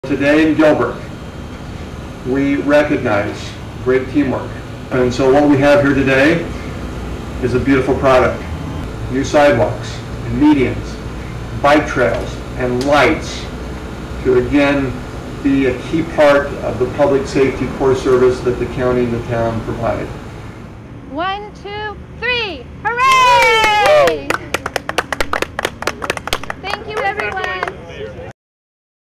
Town of Gilbert and Maricopa County DOT cut ribbon on Williams Field Rd. Project
A partnership between the Town of Gilbert and the Maricopa County DOT leads to major road improvements on Williams Field Rd.  Workers expanded a 1.5-mile section of the road, installed curbs, sidewalks, storm drains, street lighting and landscaping.